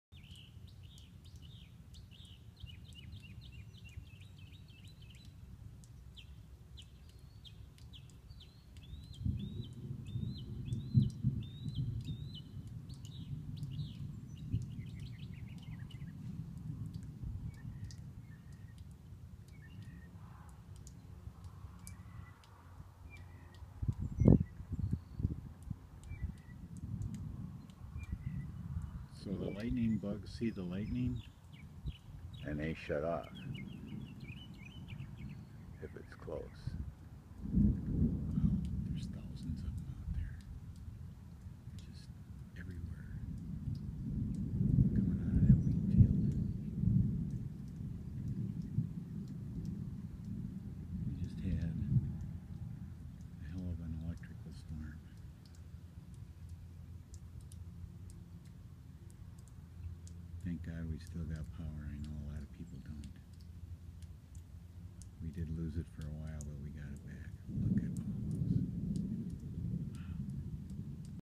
The Tesla Dance
Watch the Fireflies (you can't see all of them, but there were hundreds, maybe thousands) dance with the light show from the storms that night.